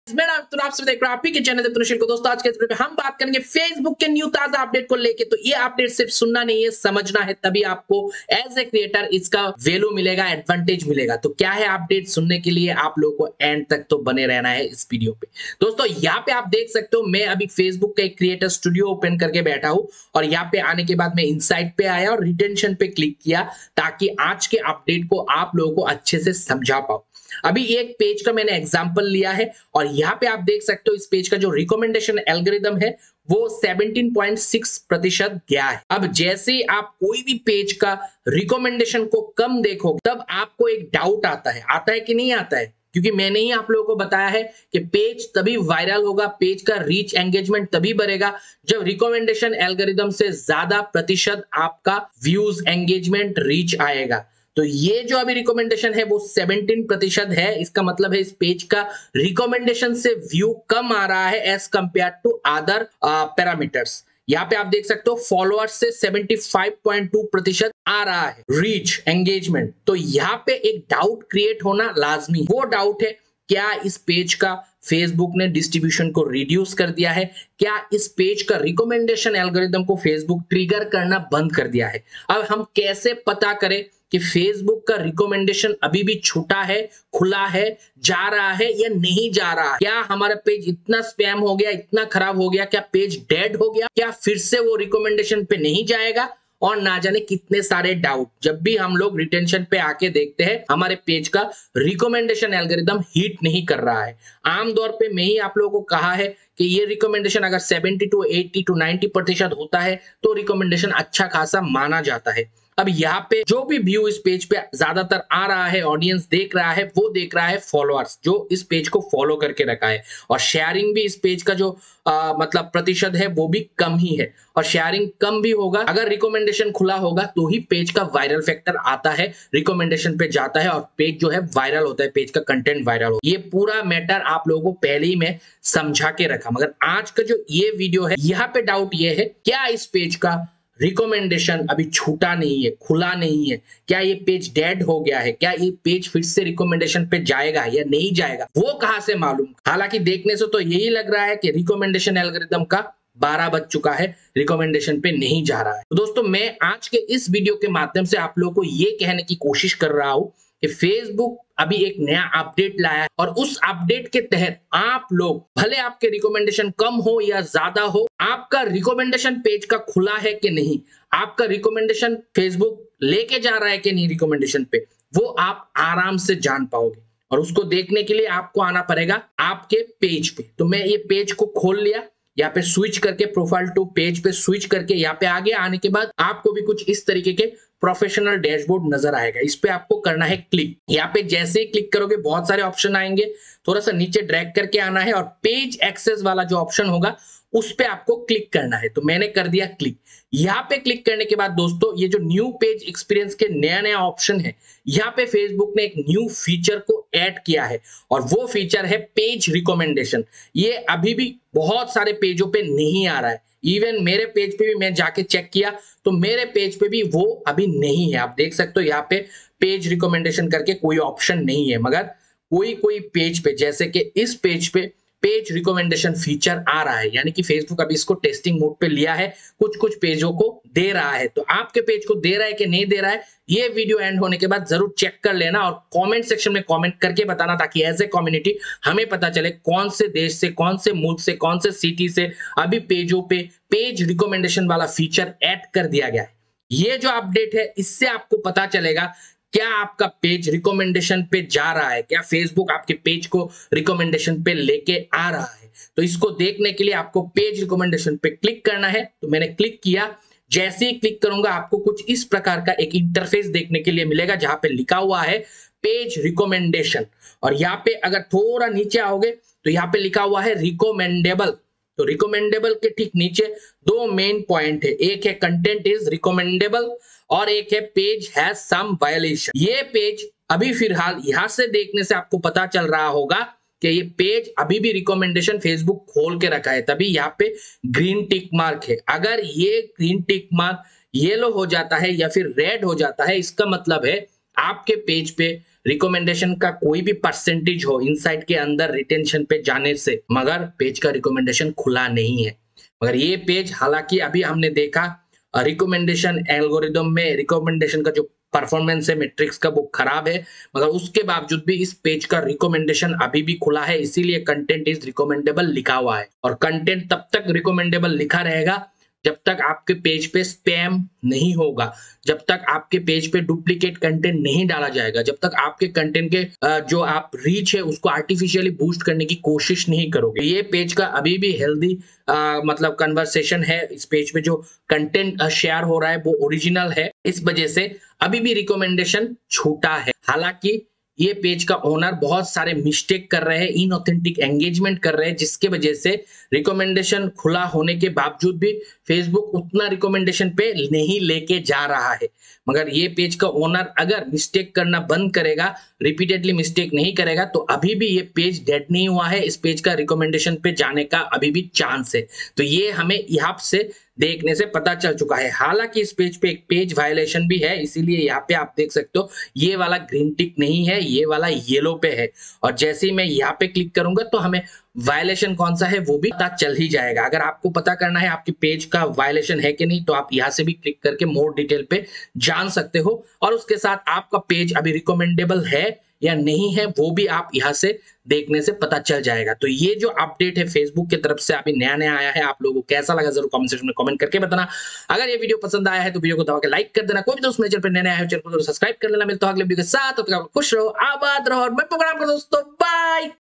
Facebook new update 2023 | Facebook Page Recommendation | By Diptanu Shil
Facebook new update 2023 | Facebook Page Recommendation | Facebook update 2023 | By Diptanu Shil // Facebook now come up with new Facebook update 2023 and this Facebook new feature help creator to understand Facebook page recommendation algorithm clearly!

Recommendable
We're helping you grow your audience because Pages like yours bring people together. Your Page is now in lists where we suggest Pages to new People.
Content is recommendable
Starting healthy conversations and sharing unique content means we're showing your Page to new people.
Page has some violations
You have your first violation, it happens. Review the violations detail page to learn how you can keep your good standing.